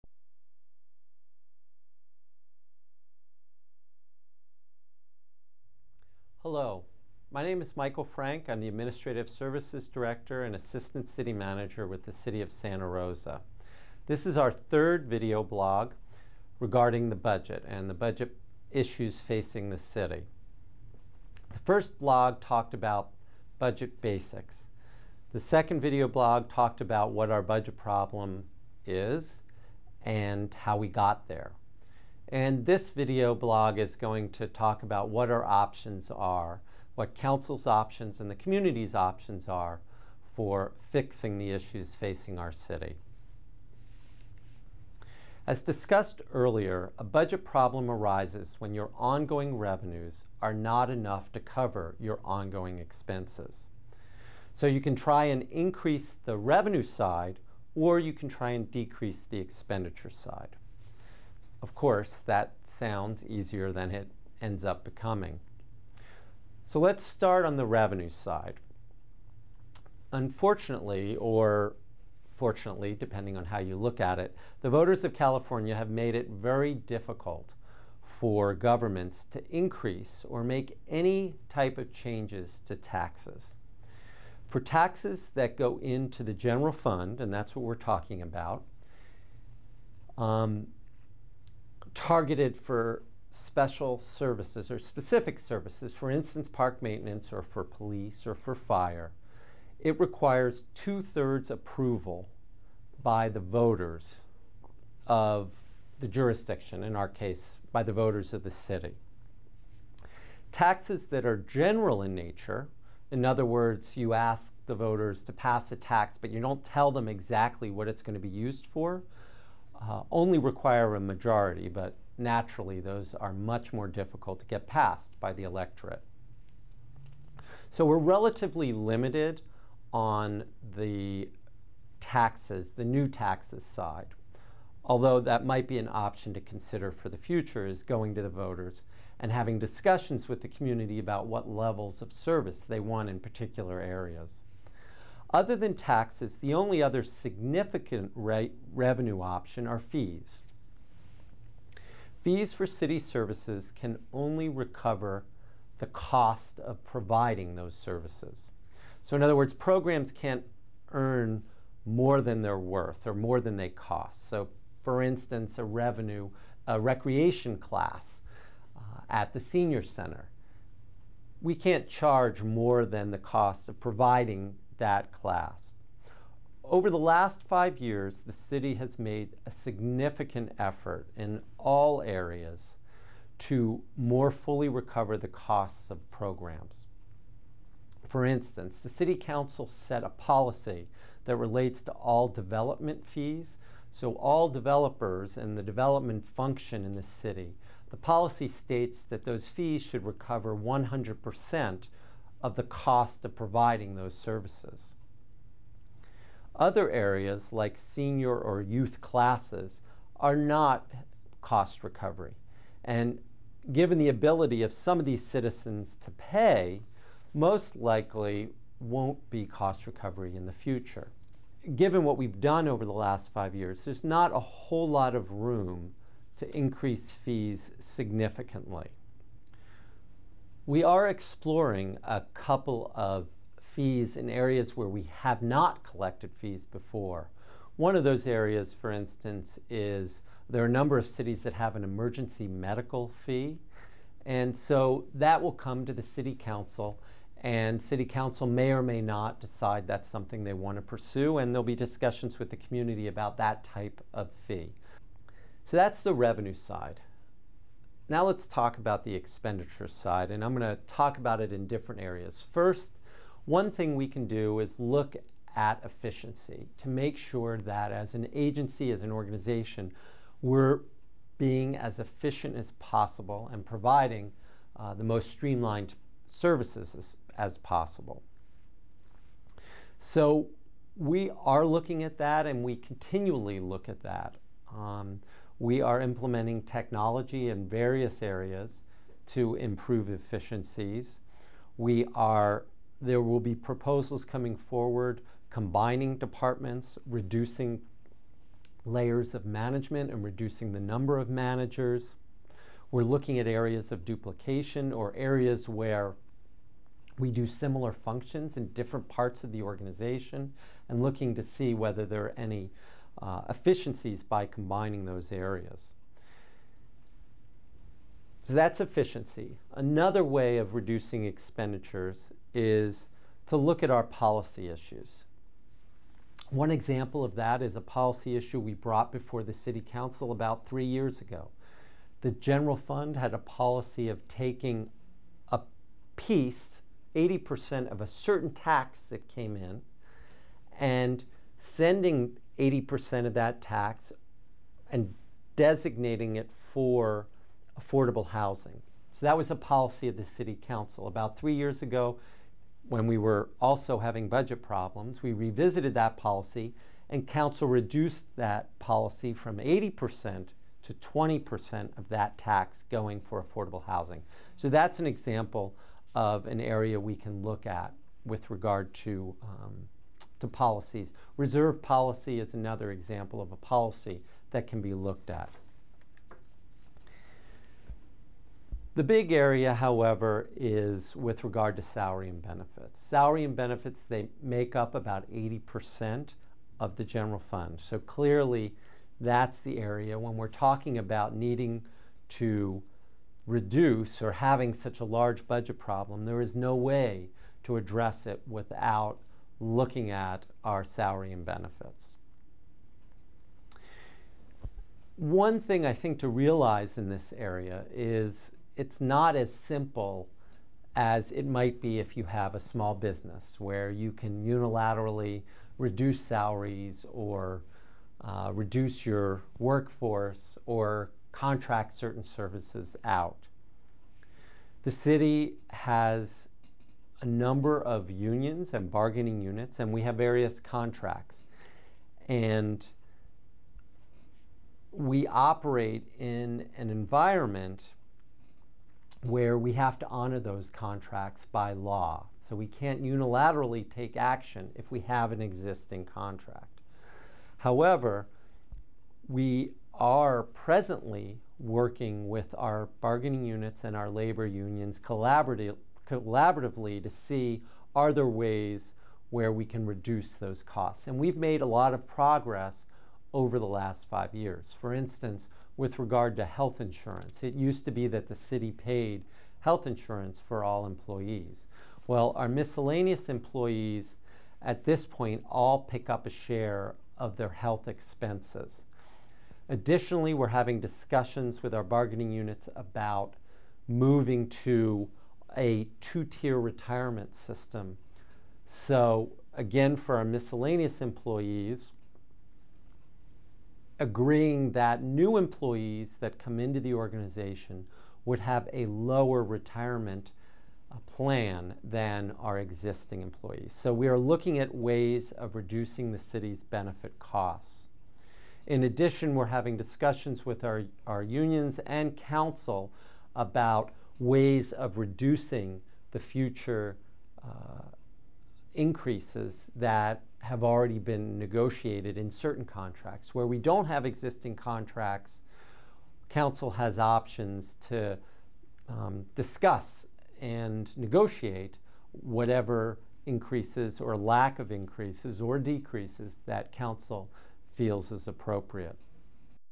City of Santa Rosa Budget Options and Solutions Part1 - January 2009
Part 1 of Asst City Mgr Michael Frank's vblog on Santa Rosa's budget crisis